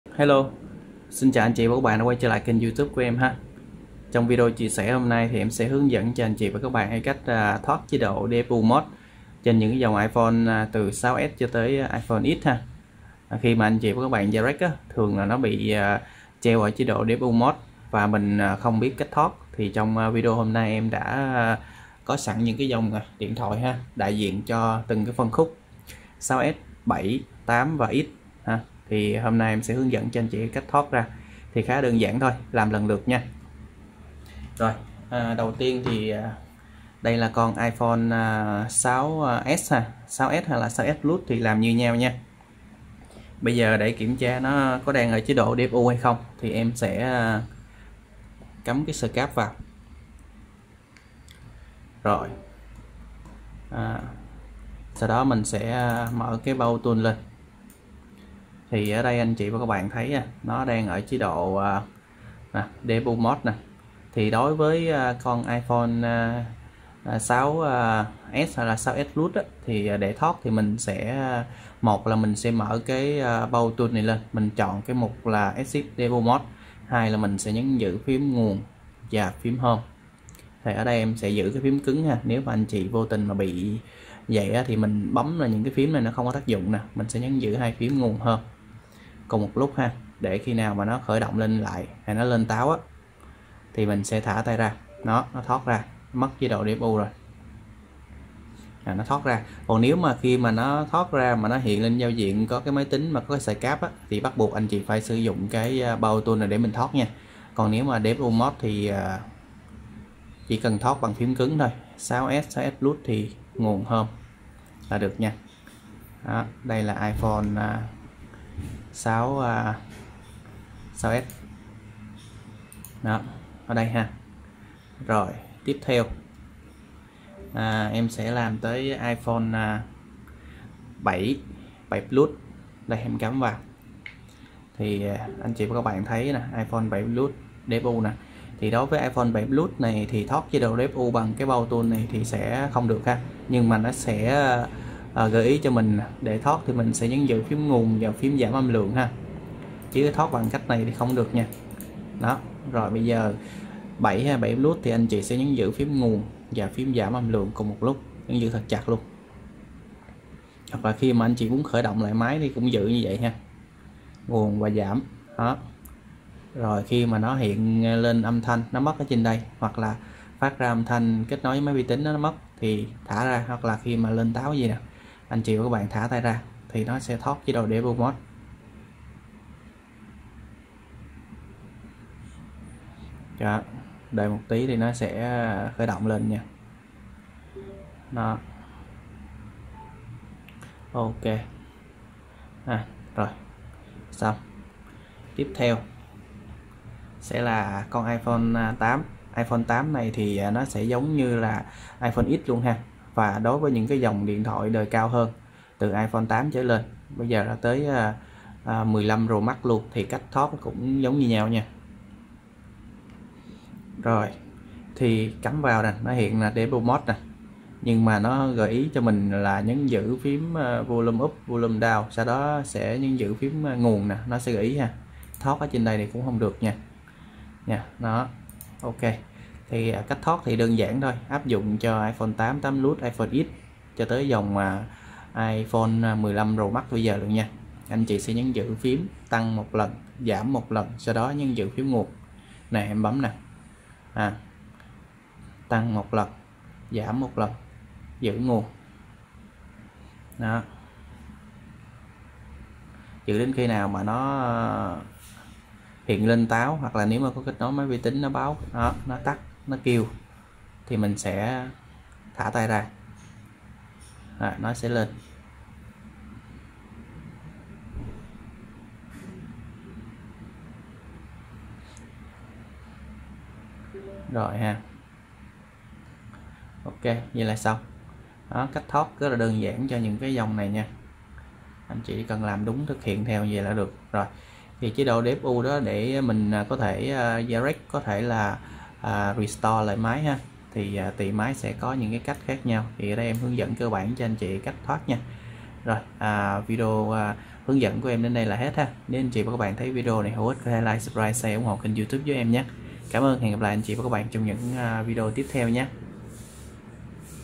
Hướng Dẫn Cách Thoát DFU Mode Trên iPhone 6S - 15 Pro Max
- Xin chào các bạn trong video home nay mình xin chia sẻ đến các bạn video "Hướng Dẫn Cách Thoát DFU Mode Trên iPhone 6S - 15 Pro Max"

XEM THÊM